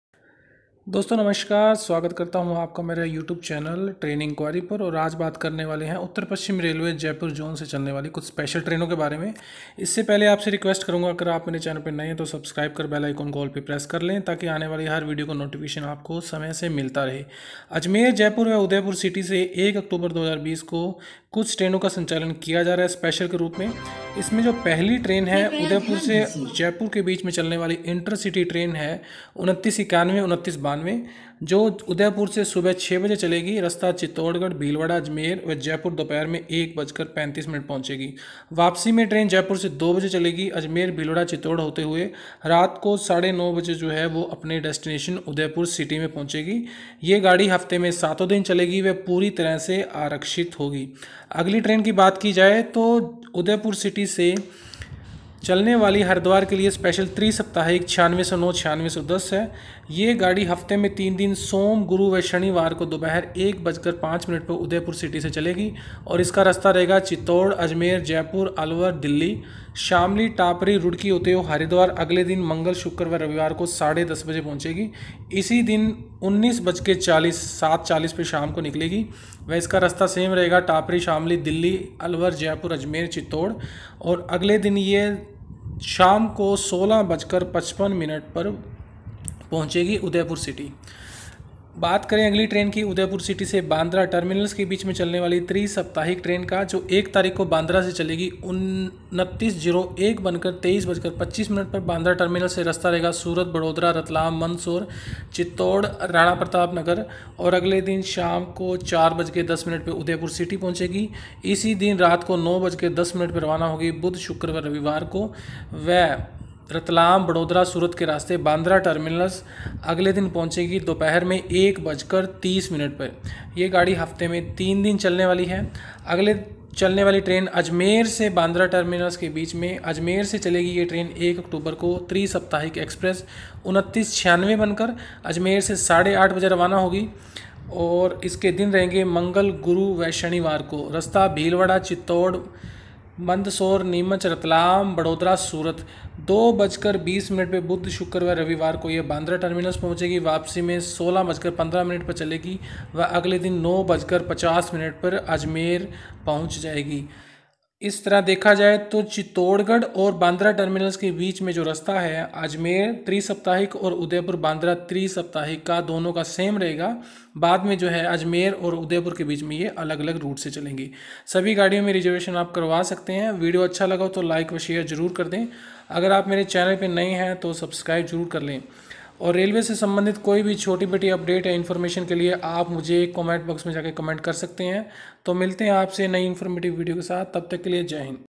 Jaipur Udaipur intercity! !Udaipur-Haridwar exp.!BandraT -Udaipur exp. Ajmer bandraT exp. from 1 oct
4 Train in NWR will starts from jaipur ajmer udaipur city.
jaipur udaipur intercity.
udaipur haridwar exp.
Ajmer -Bandra T
Udaipur- Bandra T
covid train.
train kab chalegi